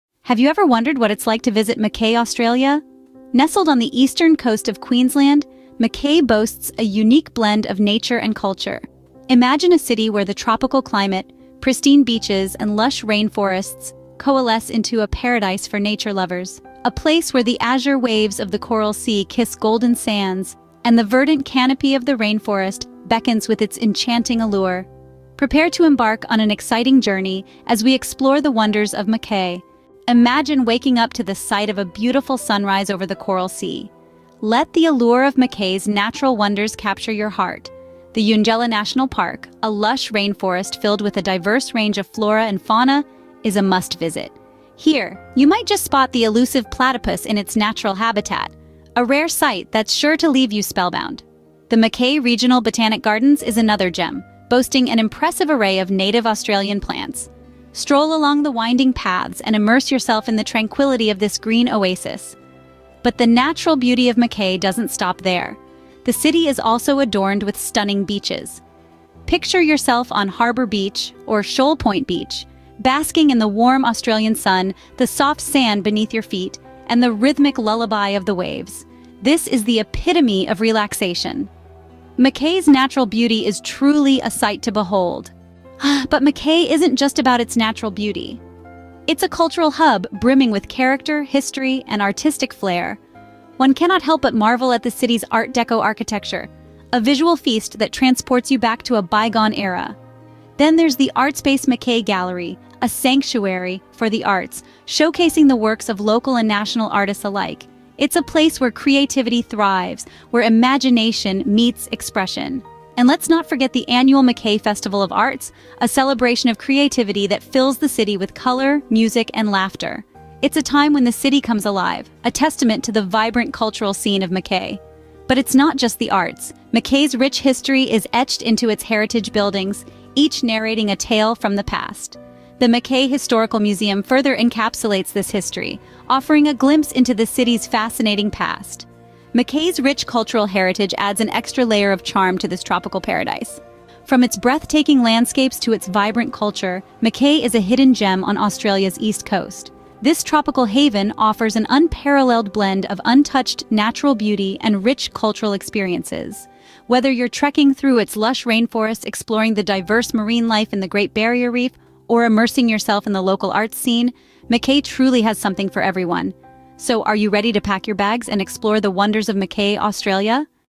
The Magic of Mackay: Exploring Its Stunning Beaches and Rainforests (3 Minutes)
In this enchanting video, we present "Discover Mackay Magic: Australia’s Hidden Gem Awaits You," where we take you on a journey through one of Australia’s most captivating towns. Nestled in Queensland, Mackay is renowned for its stunning beaches, lush rainforests, and vibrant local culture. Join us as we explore the breathtaking landscapes, including the beautiful Eungella National Park and the pristine shores of the Whitsunday Coast. We will also highlight local attractions, such as the Mackay Regional Botanic Gardens and the lively arts scene that showcases the town's creativity. Whether you're an adventure seeker, a nature lover, or simply looking for a unique travel destination, this guide will provide valuable insights and inspiration to help you experience the magic of Mackay. Get ready to uncover the beauty of this hidden gem in Australia!